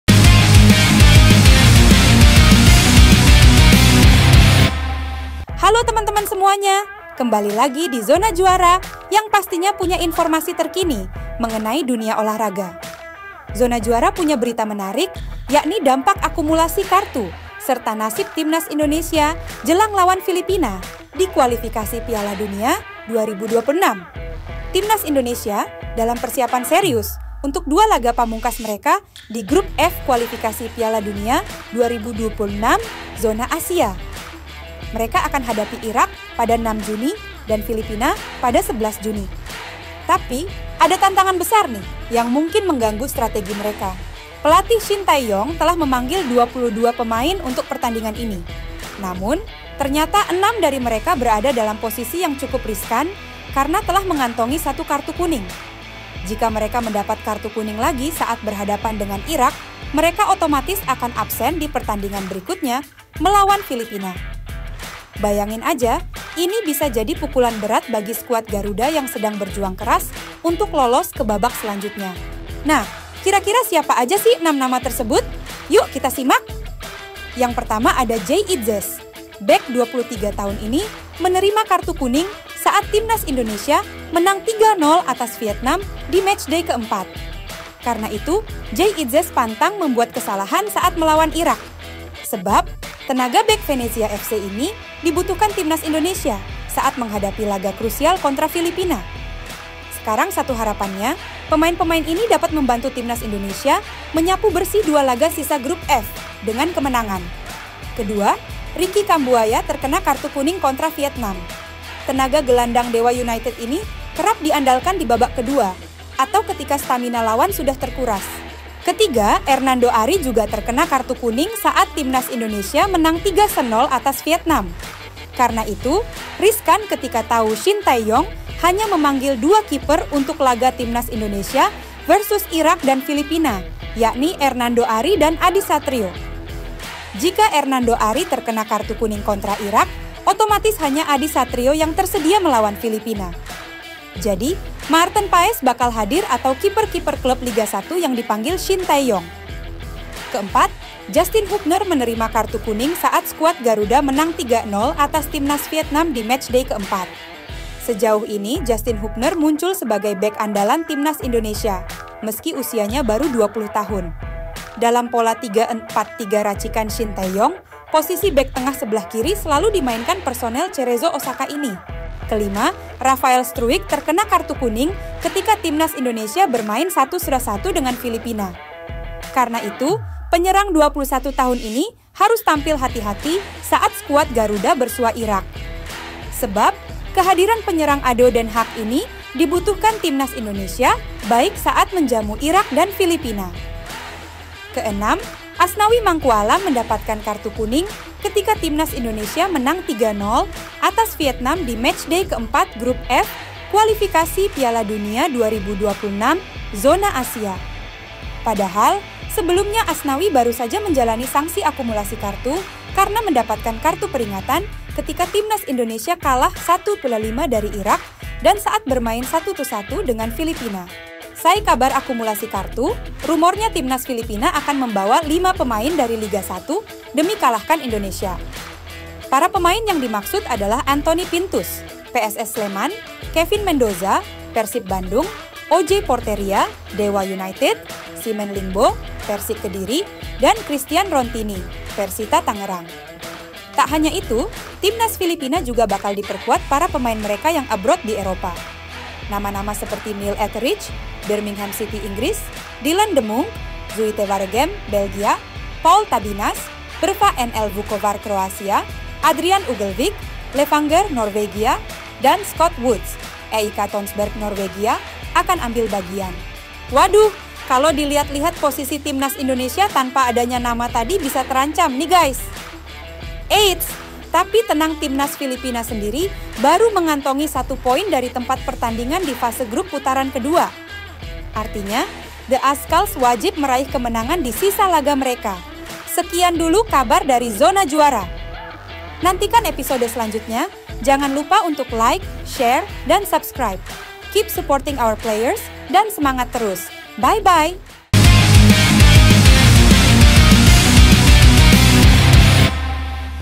6 PEMAIN TIMNAS INDONESIA INI AKAN ABSEN DI KUALIFIKASI PIALA DUNIA? - ZONA JUARA
Timnas Indonesia dalam persiapan serius untuk dua laga pamungkas mereka di Grup F Kualifikasi Piala Dunia 2026 zona Asia. Mereka akan hadapi Irak pada 6 Juni dan Filipina pada 11 Juni. Tapi, ada tantangan besar nih yang mungkin mengganggu strategi mereka.

Pelatih Shin Tae-yong telah memanggil 22 pemain untuk pertandingan ini. Namun, ternyata enam dari mereka berada dalam posisi yang cukup riskan karena telah mengantongi satu kartu kuning.

Jika mereka mendapat kartu kuning lagi saat berhadapan dengan Irak, mereka otomatis akan absen di pertandingan berikutnya melawan Filipina. Bayangin aja, ini bisa jadi pukulan berat bagi skuad Garuda yang sedang berjuang keras untuk lolos ke babak selanjutnya.